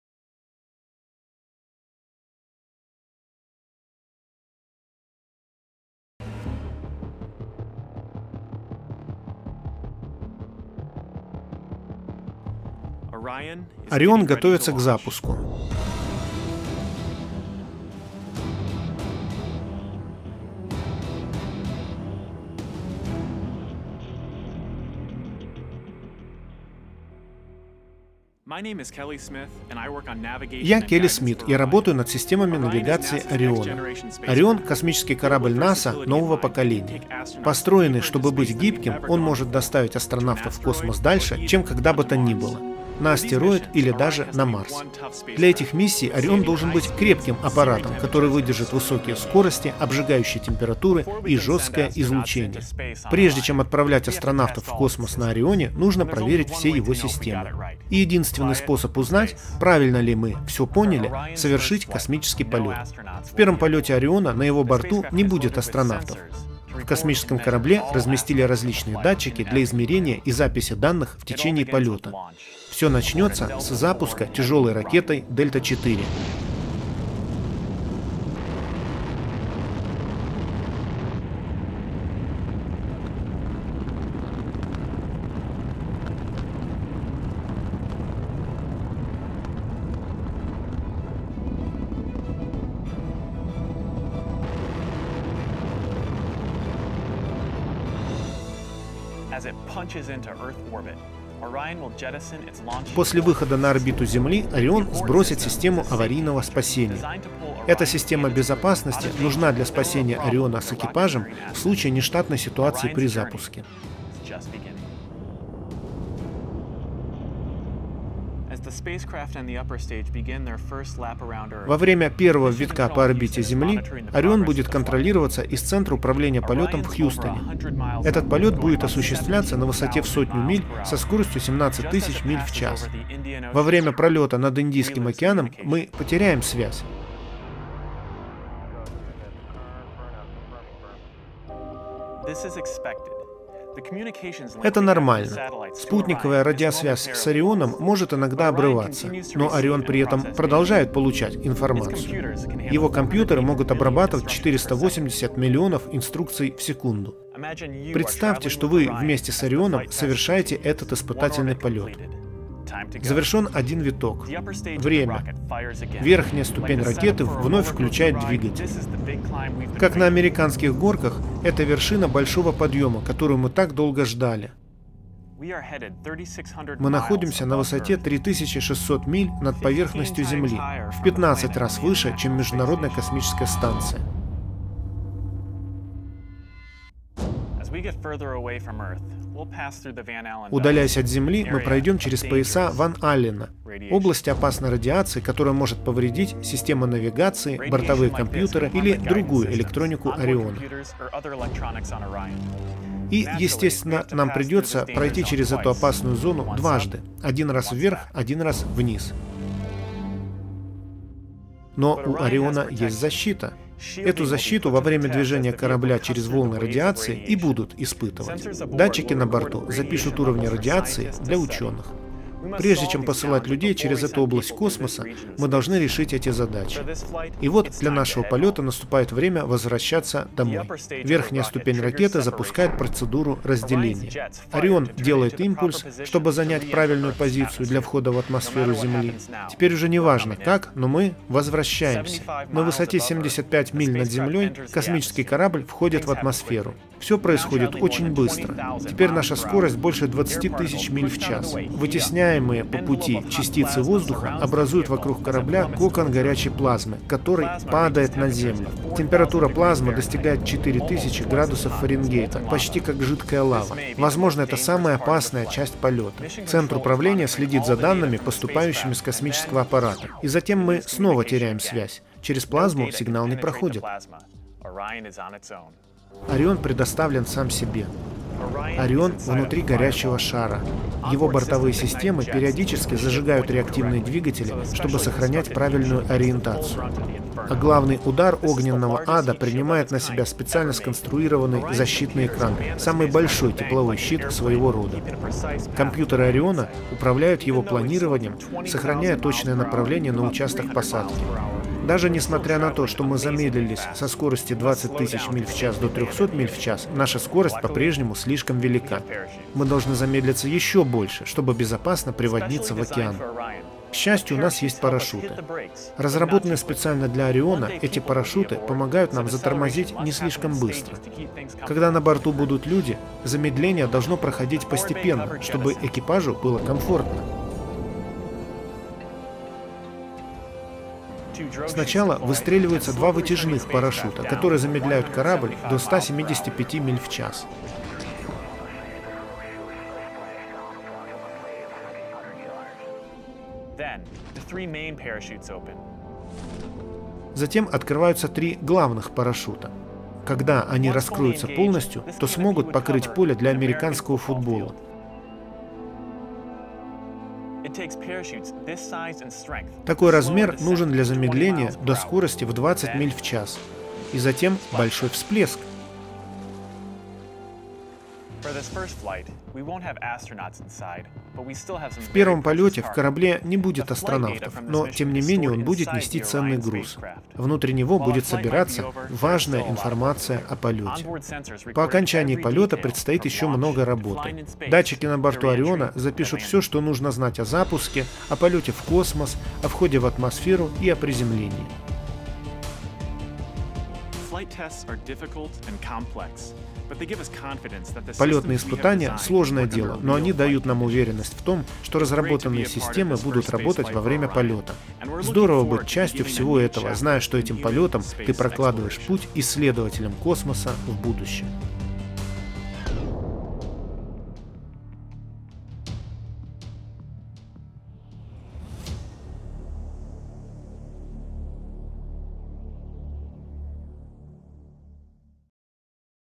NASA | Orion | Испытание огнем
Сегодня запускали новый американский корабль "Орион". По причине неисправности в дренаже клапанов ракеты Дельта, запуск отложен до завтра.

Ролик рассказывает о плане этого полета